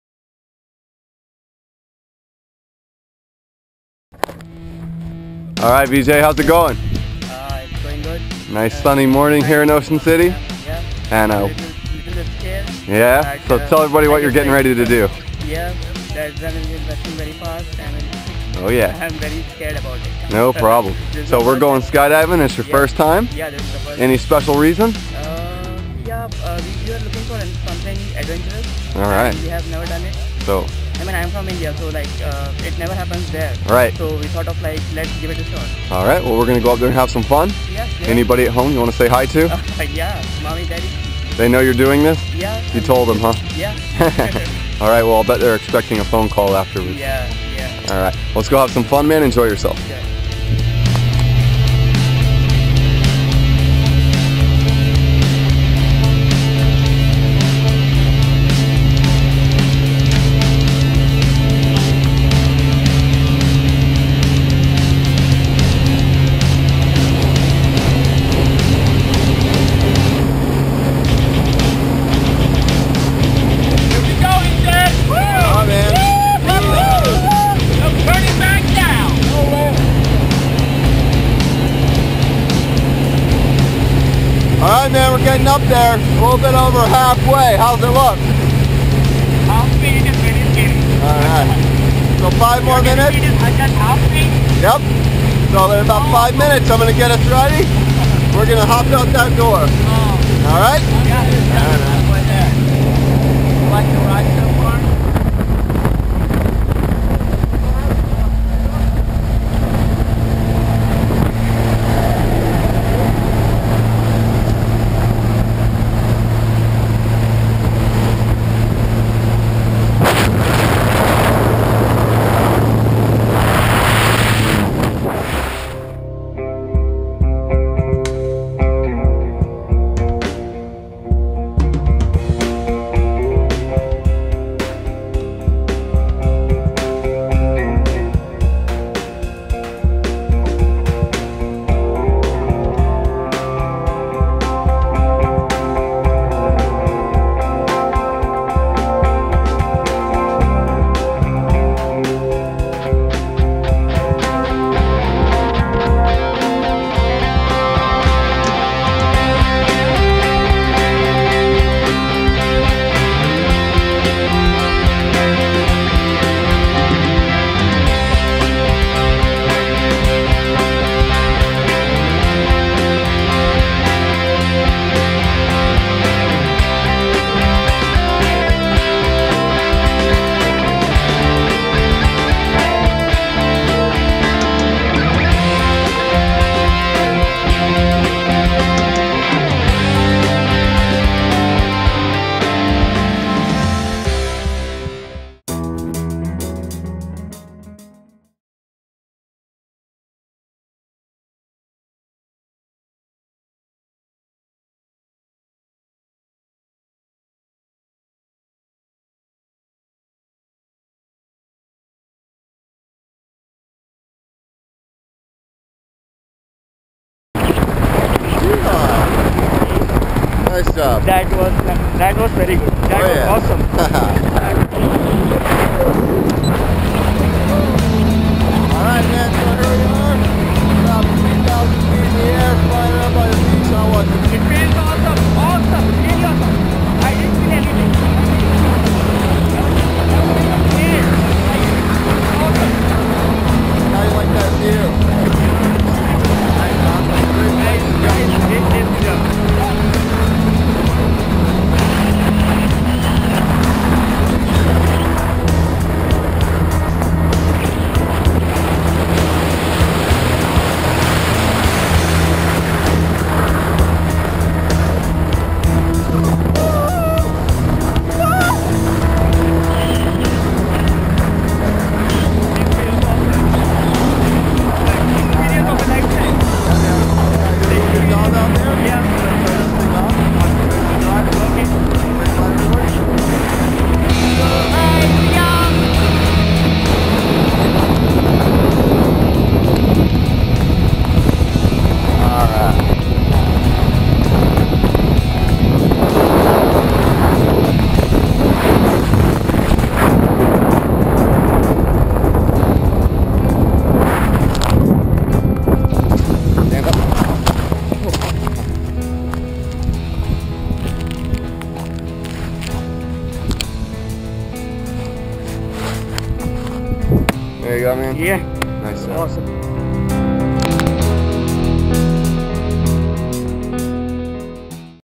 Vijay Singh
Tandem Skydiving in Maryland over Ocean City - Vijay Singh